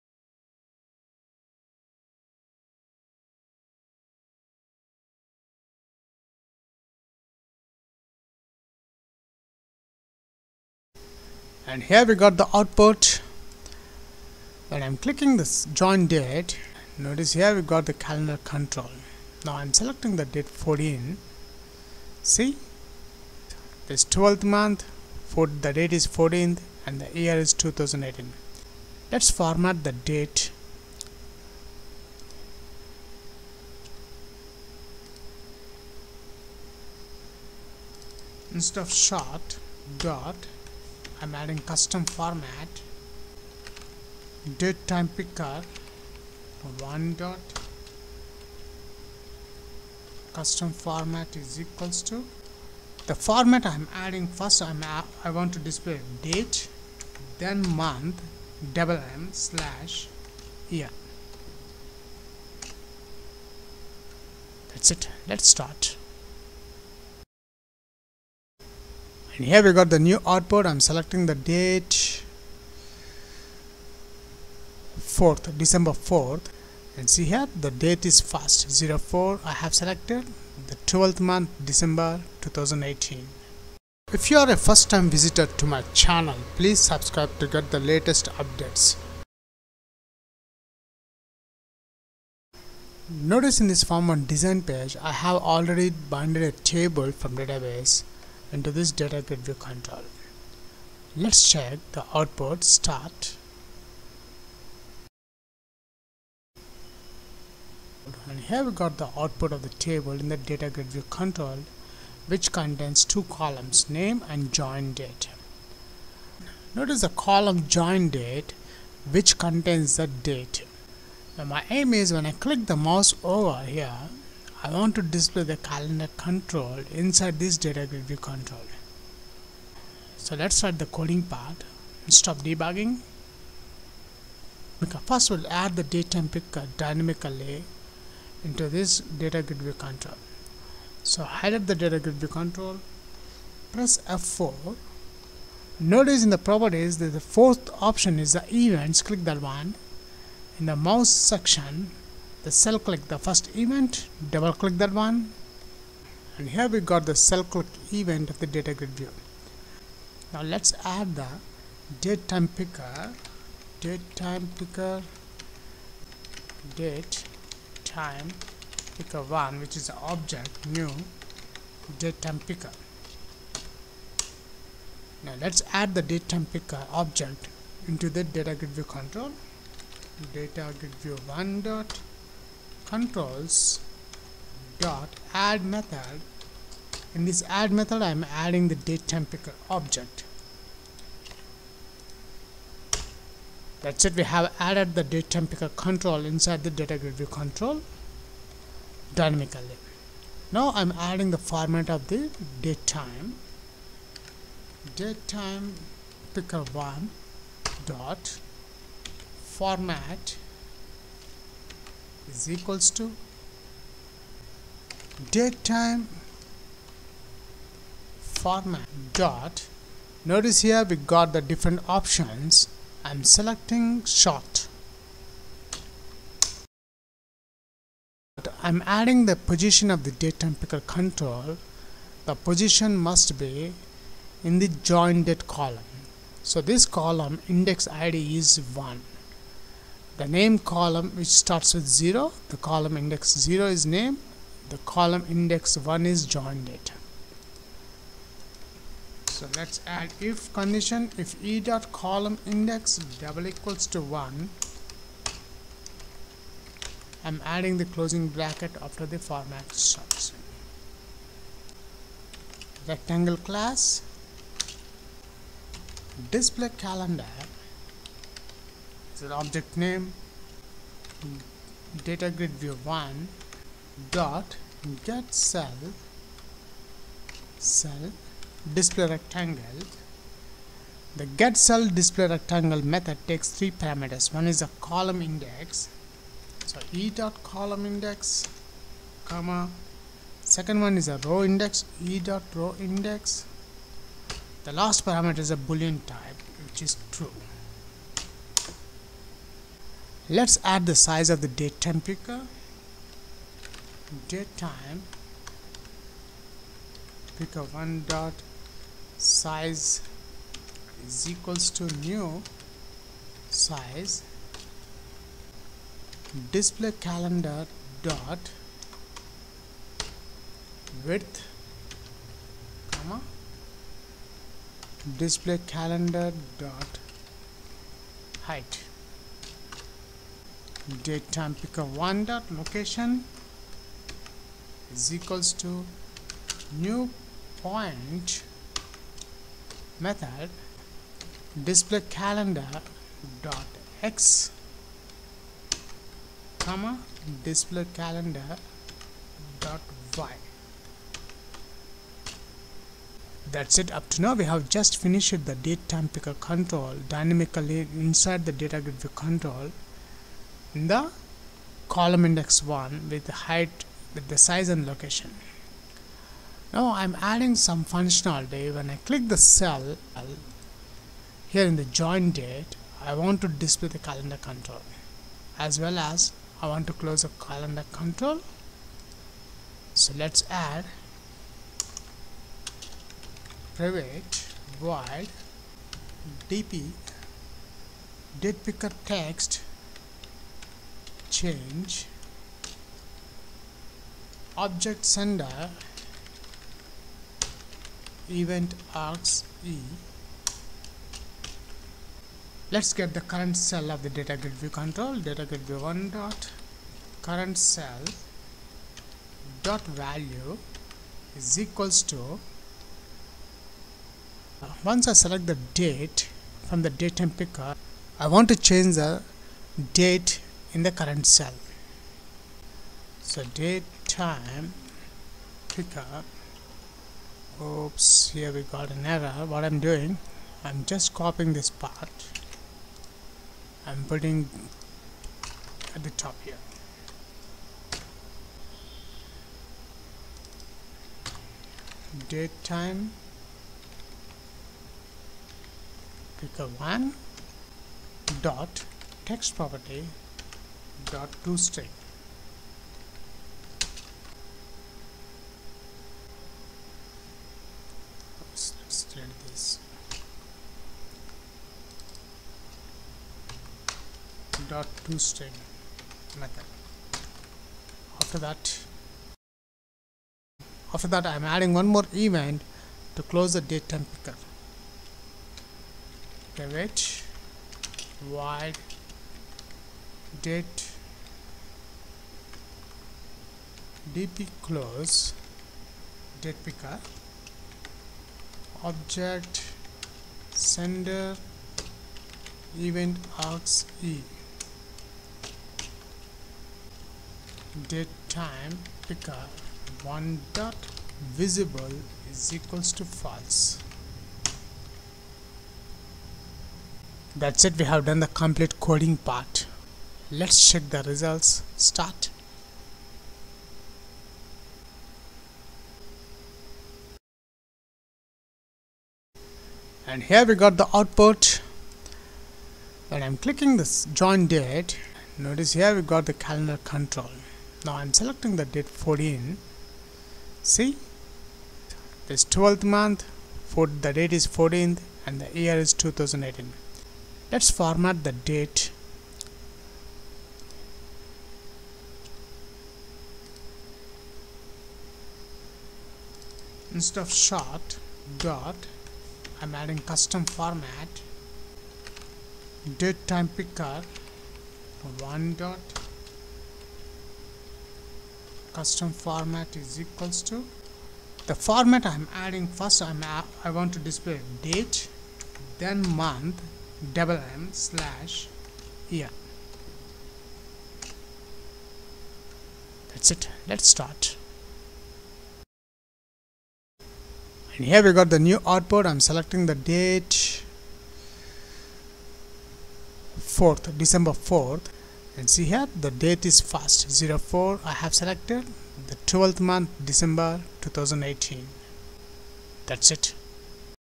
datetimepicker winforms format and display add in datagridview
adding display the winforms datetimepicker into datagridview column row cells when click and change the selected the date using c#